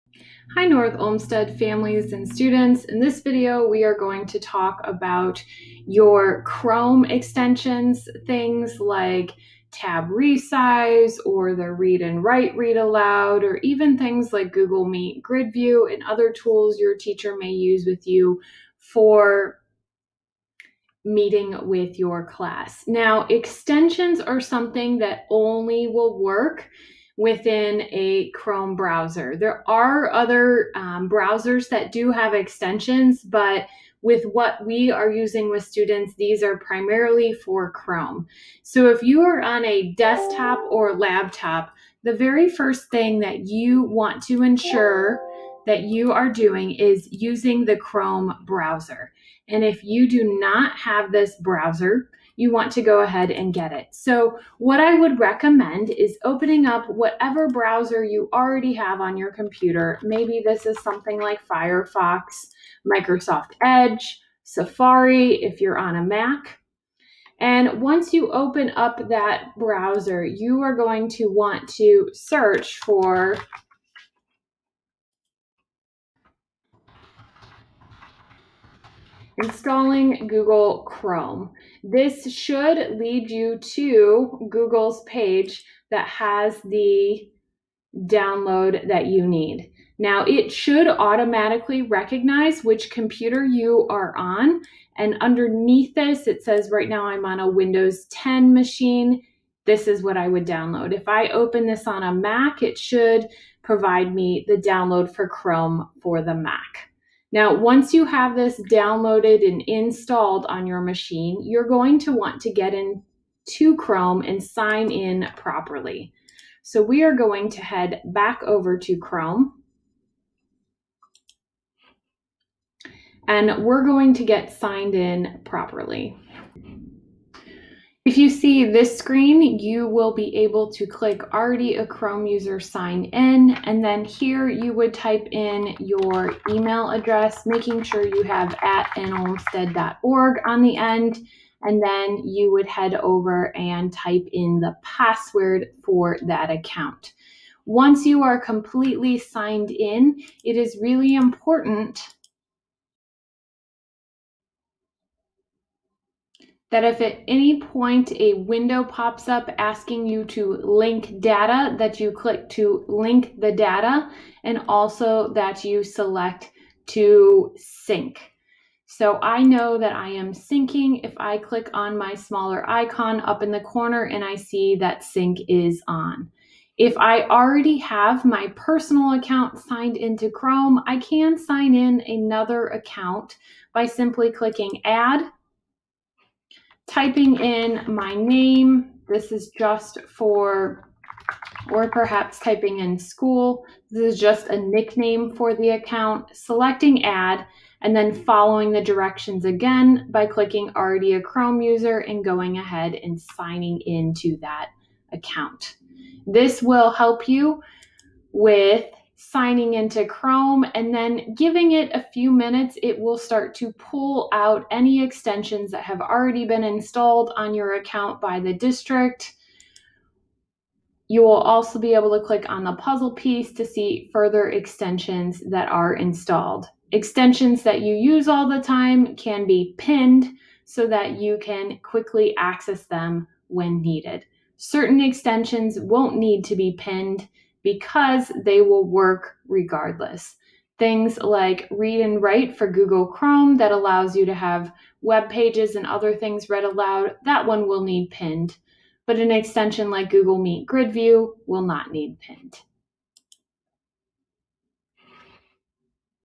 Troubleshooting: Accessing Extensions on Desktop/Laptop
North Olmsted Families, if you are having trouble accessing extensions on your desktop or laptop, this video will help you with getting the needed access.